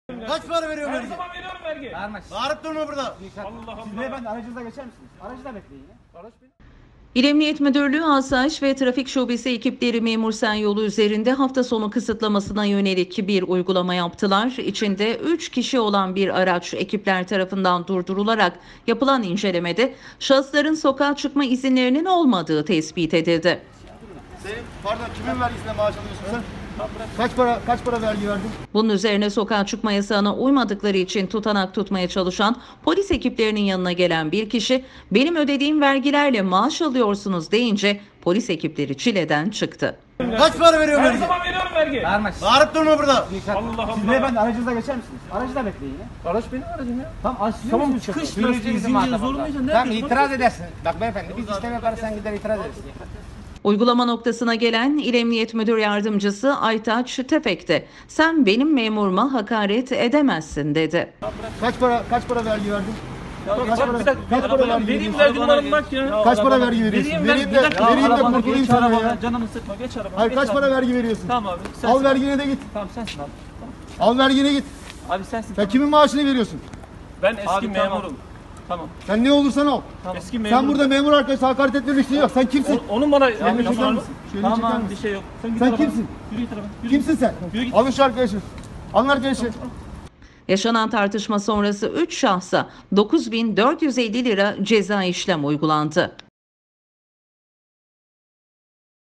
“SEN BENİM MEMURUMA HAKARET EDEMEZSİN”
Memur-Sen bağlantı yolunda hafta sonu kısıtlaması nedeniyle uygulama yapan polis ekipleri içinde 3 kişi olan bir aracı durdurdu. Araçtan inen  bir şahıs “Benim ödediğim vergilerle maaş alıyorsunuz” deyince olay yerinde bulunan İl Emniyet Müdür Yardımcısı çileden çıktı.

İl Emniyet Müdürlüğü Asayiş ve Trafik şubesi ekipleri Memur-Sen yolu üzerinde hafta sonu kısıtlamasına yönelik uygulama yaptılar.
İçinde 3 kişi olan bir araç ekipler tarafından durdurularak yapılan inceleme de şahısların sokağa çıkma izinlerinin olmadığı tespit edildi.
Bunun üzerine sokağa çıkma yasağına uymadıkları için tutanak tutmaya çalışan polis ekiplerinin yanına gelen 1 kişi “Benim ödediğim vergilerle maaş alıyorsunuz” deyince polis ekipleri çileden çıktı.

Uygulama noktasına gelen İl Emniyet Müdür Yardımcısı Aytaç Tefek’te  “Sen benim memuruma hakaret edemezsin” dedi.
Yaşanan tartışma sonrası 3 şahsa 9 bin 450 lira cezai işlem uygulandı.